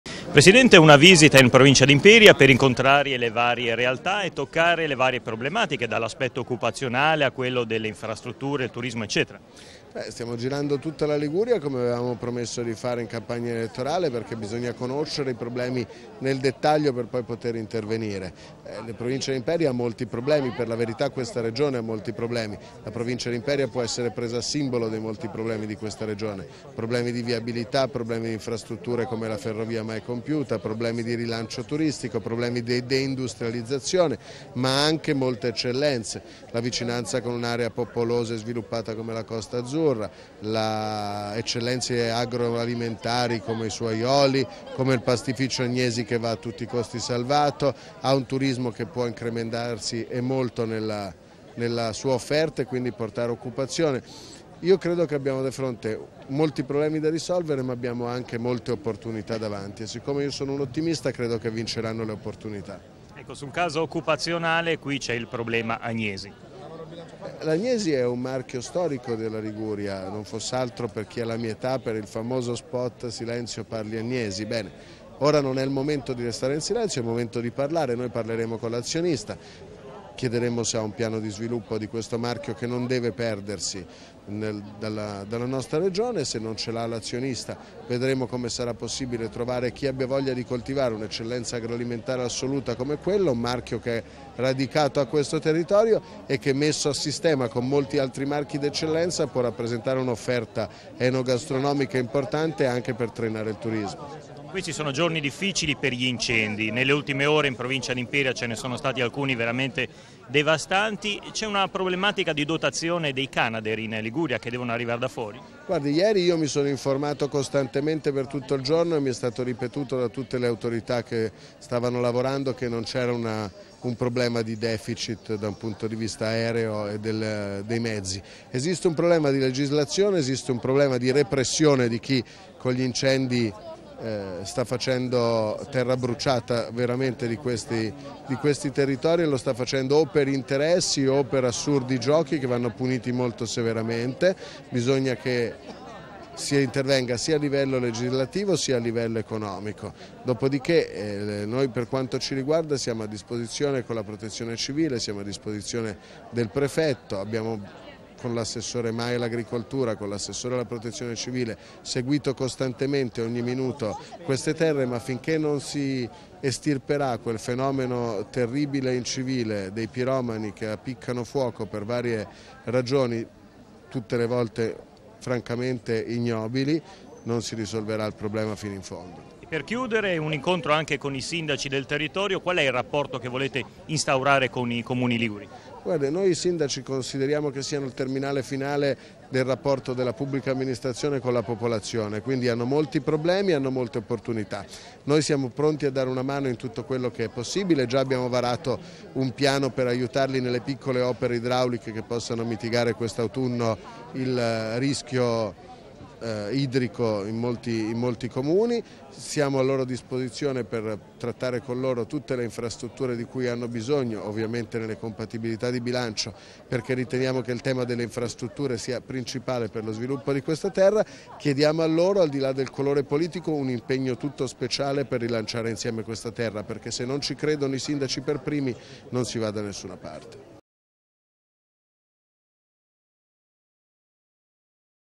PRESIDENTE DELLA REGIONE GIOVANNI TOTI IN VISITA A IMPERIA
Intervista al Presidente della Regione Giovanni Toti in occasione della sua visita di oggi a Imperia per incontrare le diverse realtà e toccare le varie problematiche che interessano la nostra provincia. Molti secondo il Presidente i problemi da risolvere, ma anche molte opportunità da sfruttare.